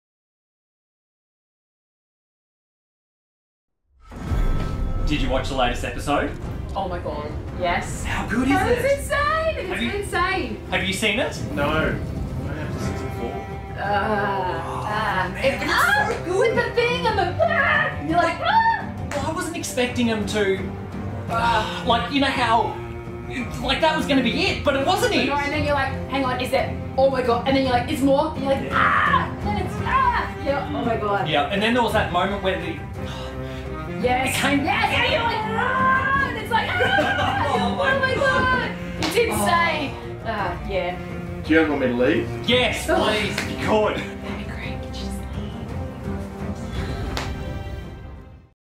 Chatting about Game of Thrones without Spoilers
Trying to chat about the latest episode of Game of Thrones in a way that doesn't spoil it for someone in the room who hasn't seen it is no easy task